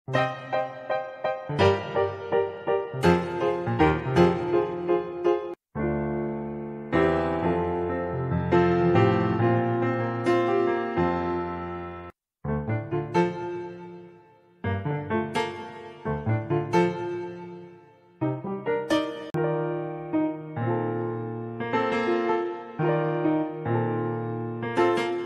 Midi/Loop Pack 600 [Ultimate Starter Bundle for Music Producers]
Welcome to the repository of midi files and loops I have produced over the last 6 months! Not ever I have used all of them. so enjoy!